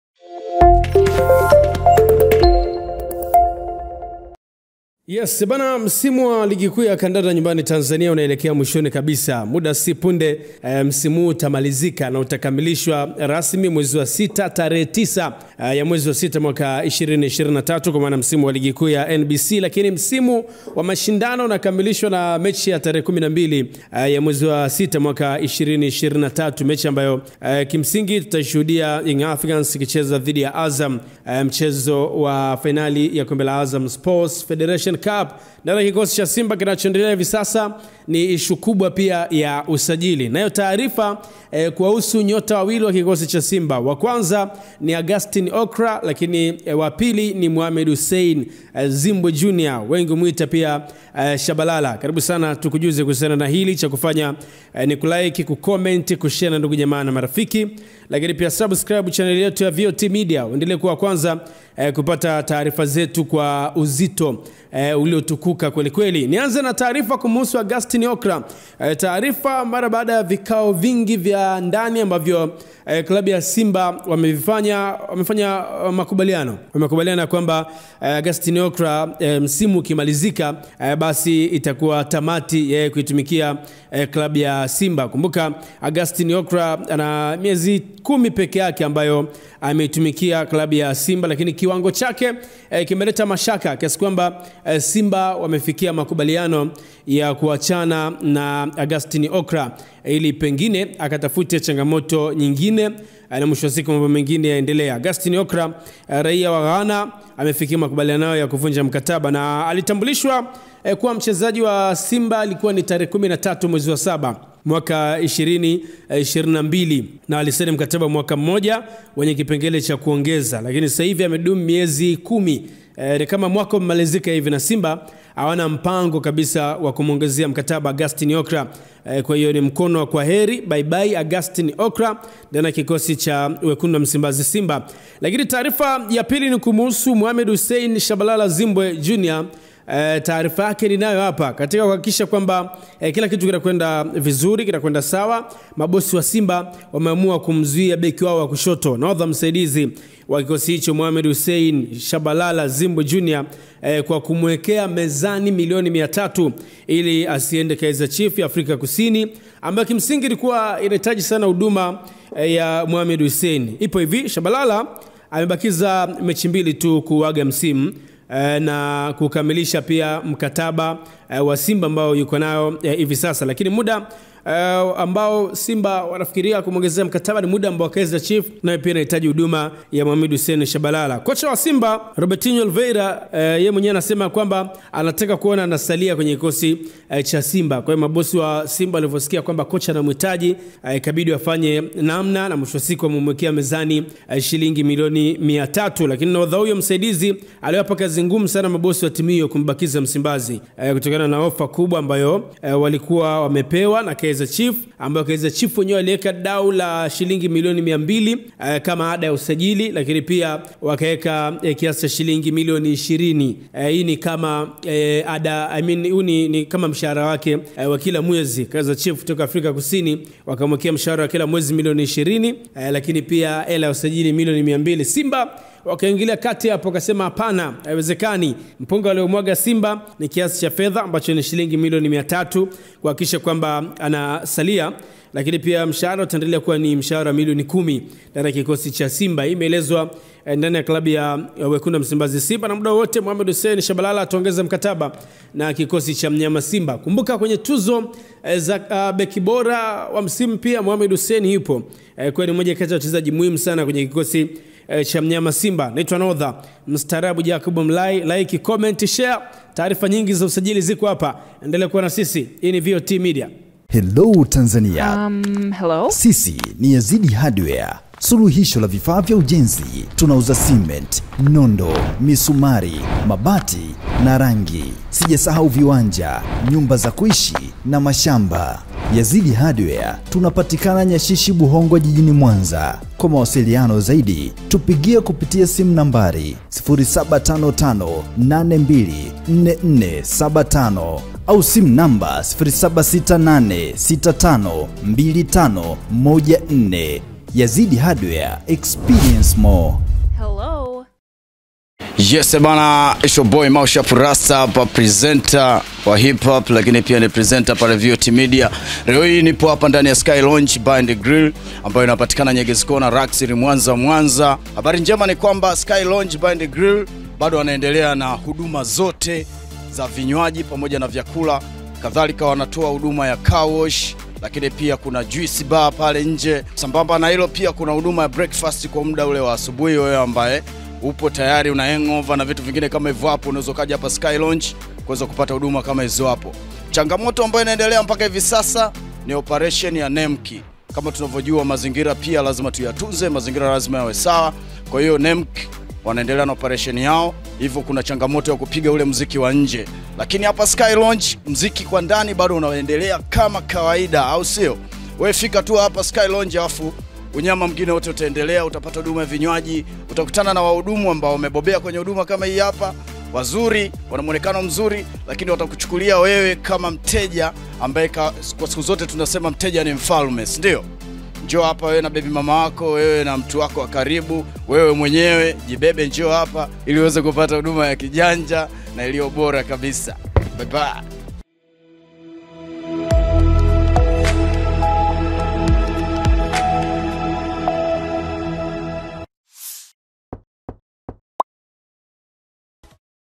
SIMBA WAMLIPA TSHABALALA MILIONI 300 AONGEZE MKATABA | OKRAH HABARI YAKE KWISHA
Hizi hapa TETESI ZA USAJILI hapa nyumbani Tanzania ambapo klabu ya Simba na Katika kuhakikisha kila kitu kinaenda sawa, mabosi wa Simba wameamua kumzuia beki wa kushoto na nahodha msaidizi  Mohamed Hussein ‘Tshabalala’ kwa kumwekewa Sh 300 milioni mezani ili asiende Kaizer Chiefs ya Afrika Kusini iliyokuwa inammnyemelea ili imsajili.